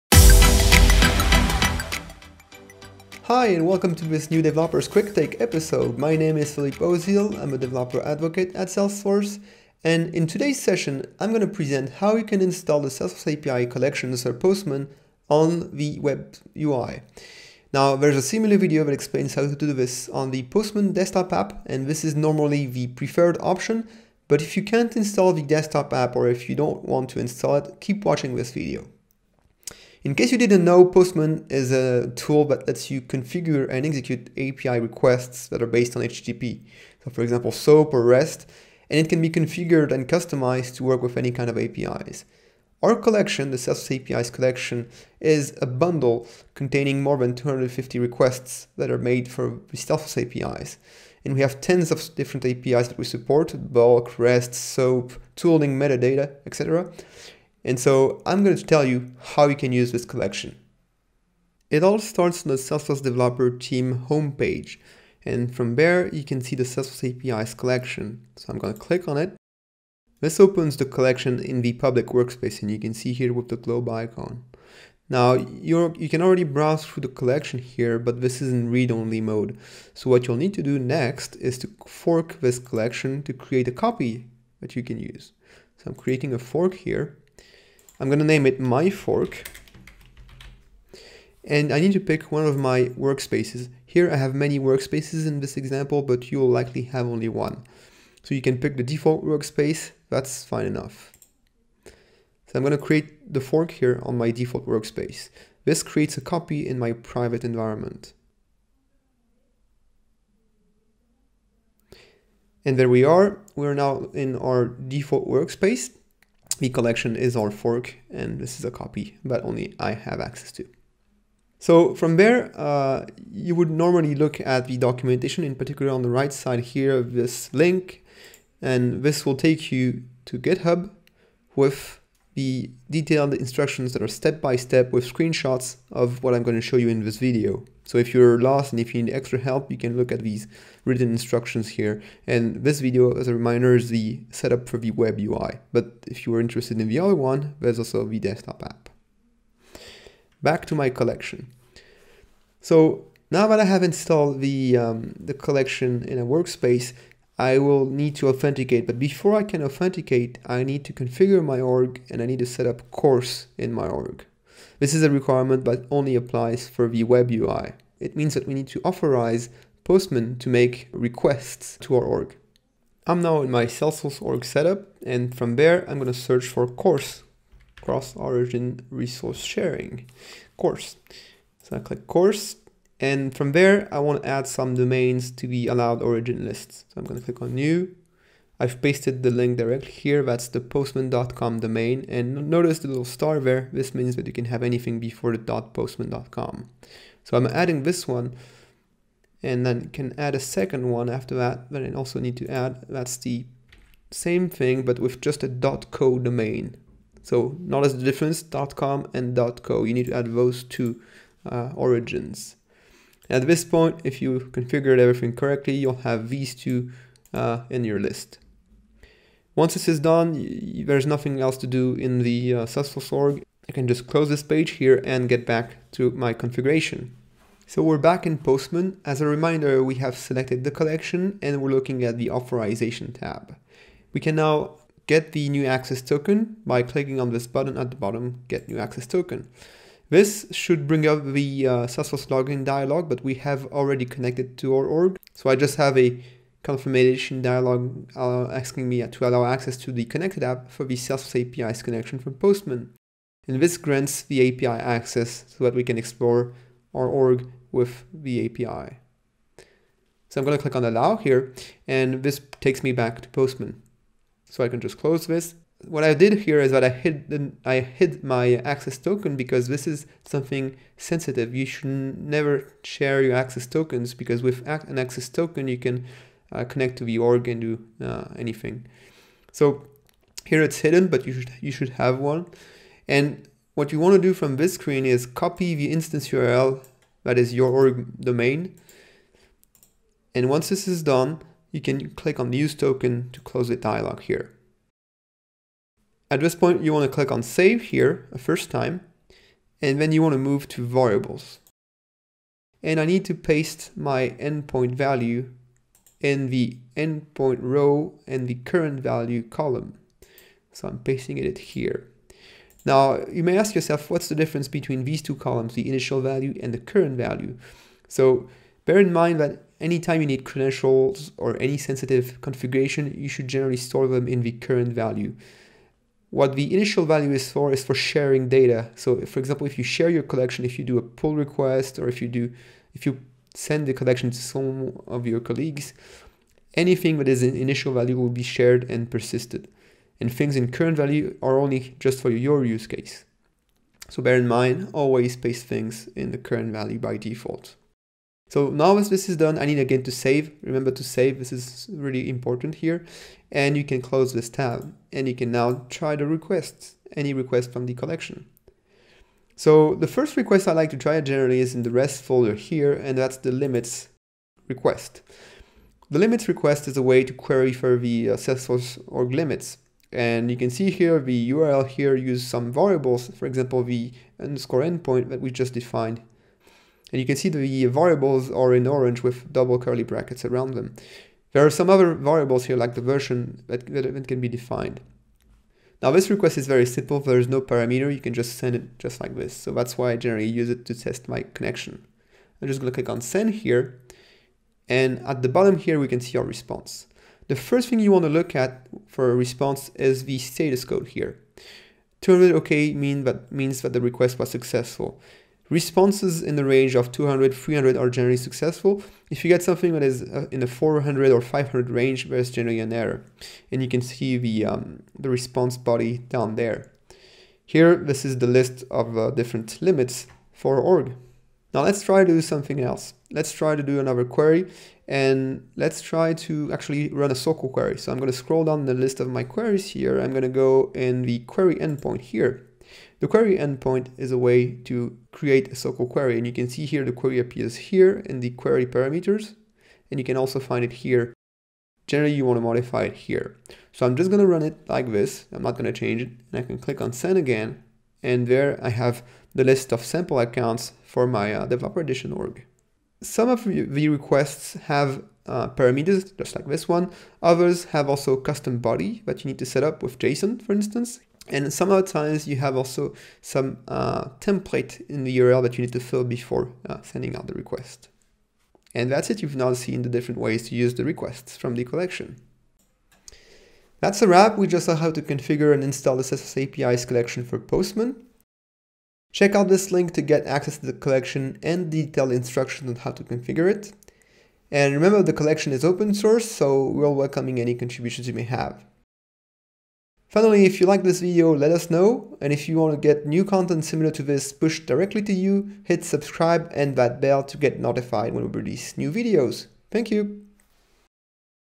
Use the Salesforce APIs Collection with the Postman Web UI | Developer Quick Takes
This video presents how you can install and use the Salesforce APIs collection using the Postman web UI.